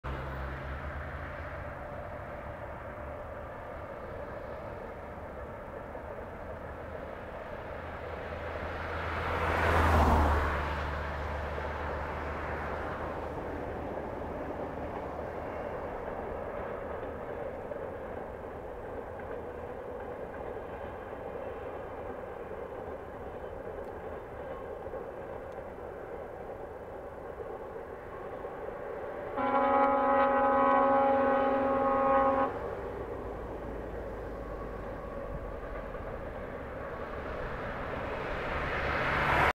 R 604 Dargov a Os 7811
Míňanie vlakov R 604 Dargov a Os 7811 zachytených na košickom zhlaví pred stanicou Spišská Nová Ves.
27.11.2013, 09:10